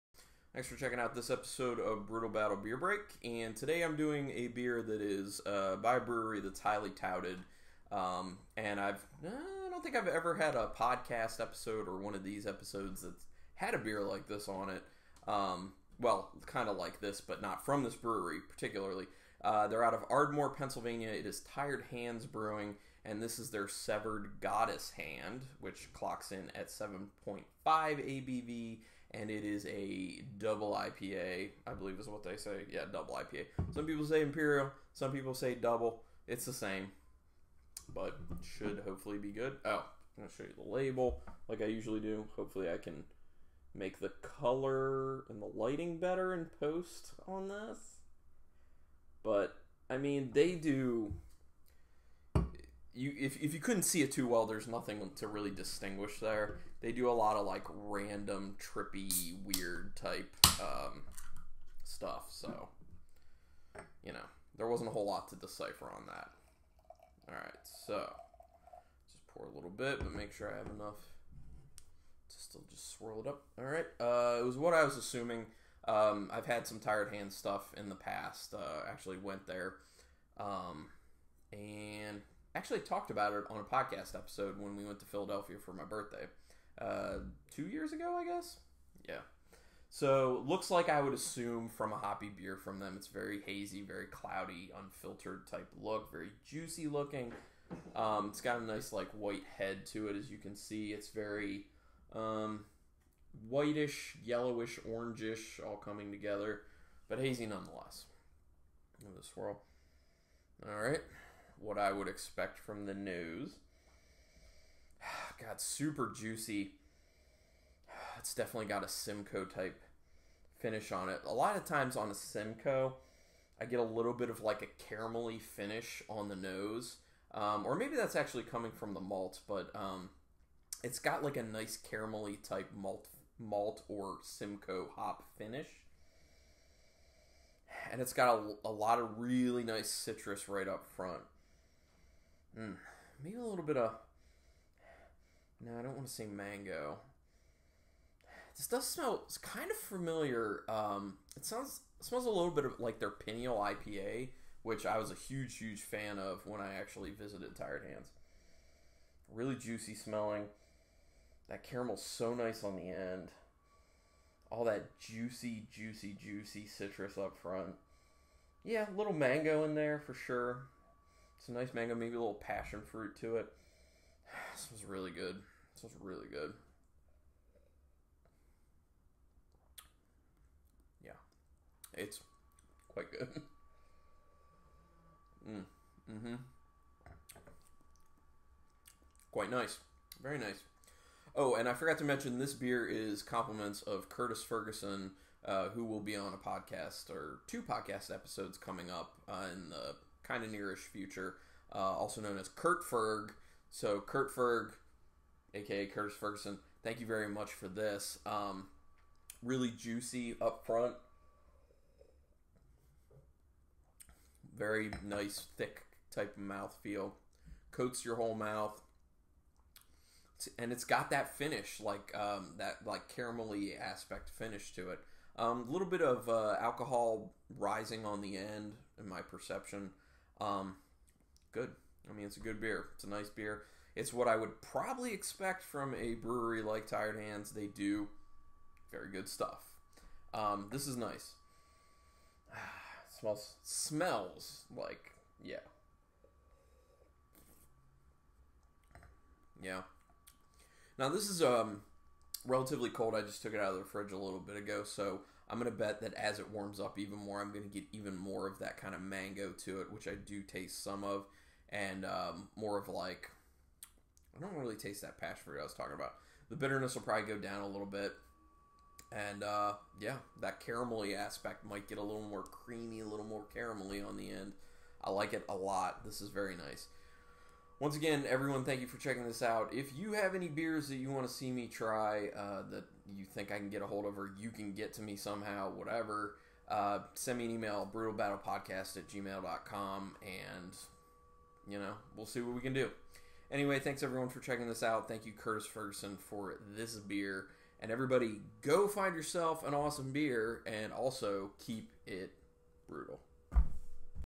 Brewtal Battle Beer Break - Severed Goddess Hand by Tired Hands Brewing (Ep. 115)
Carlin tastes Severed Goddess Hand Double IPA by Tired Hands Brewing our of Ardmore, Pennsylvania.